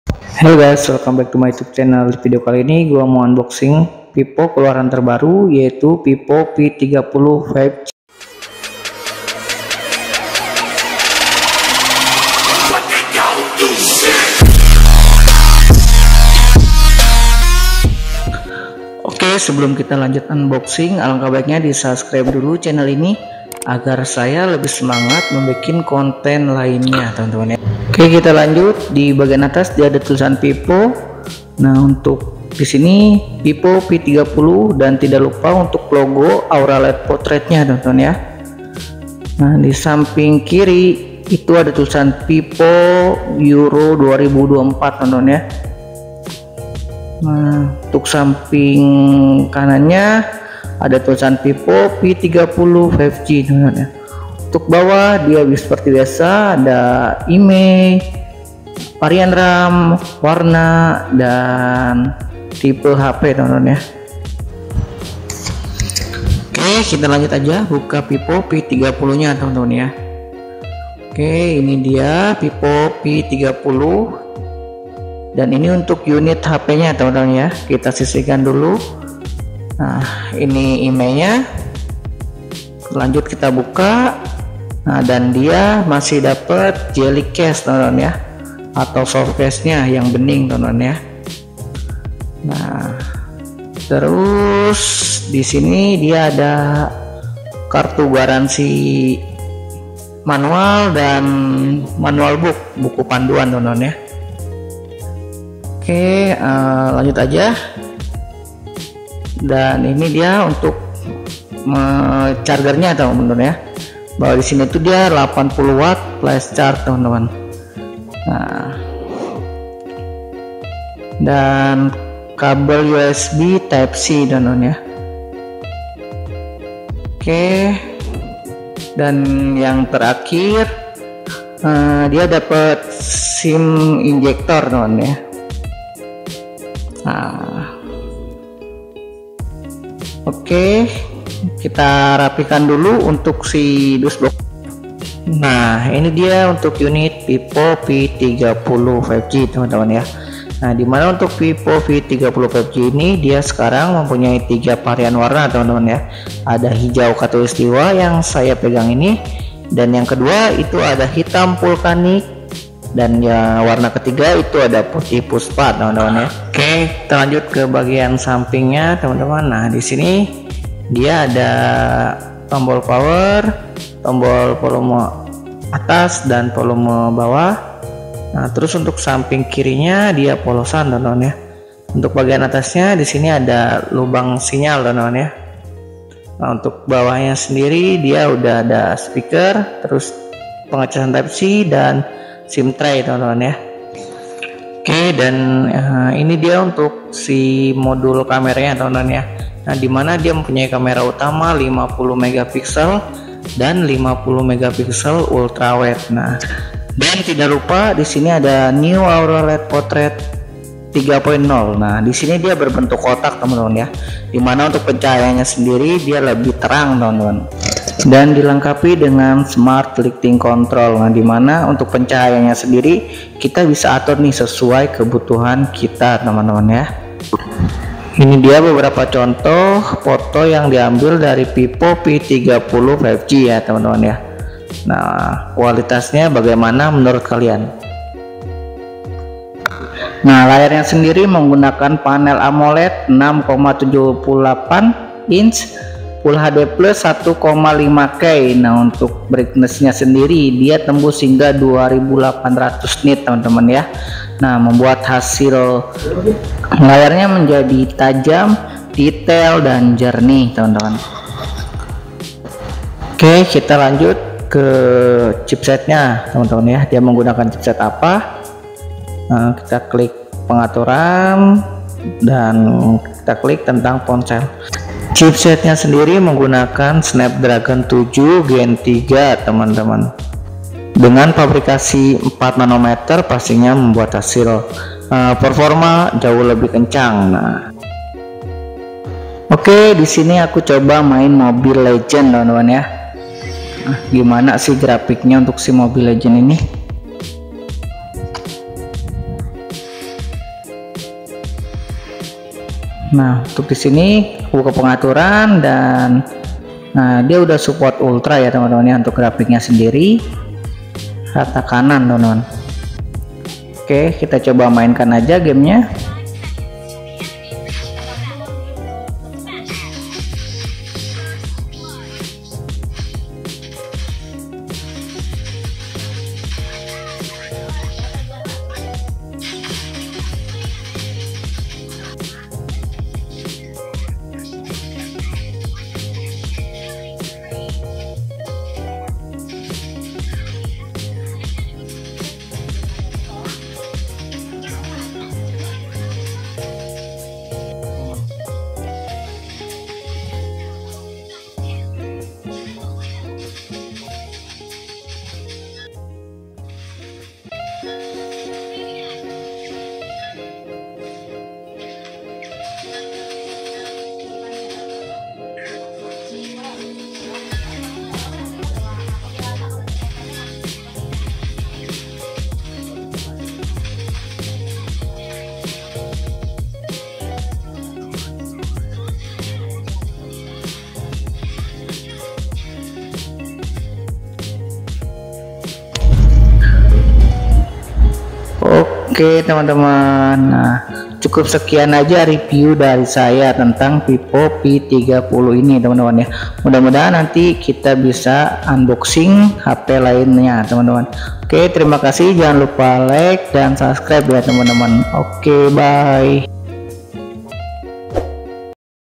RESMI RILIS UNBOXING VIVO V30
Akhir nya V30 tiba dengan chipset terbaruemakai snapdragon 7 gen3 dengan pabrikasi 4nm performa mantap dan tidak lupa new aura light portrait 3.0 membuat pencahayaan foto menjadi lebih mantap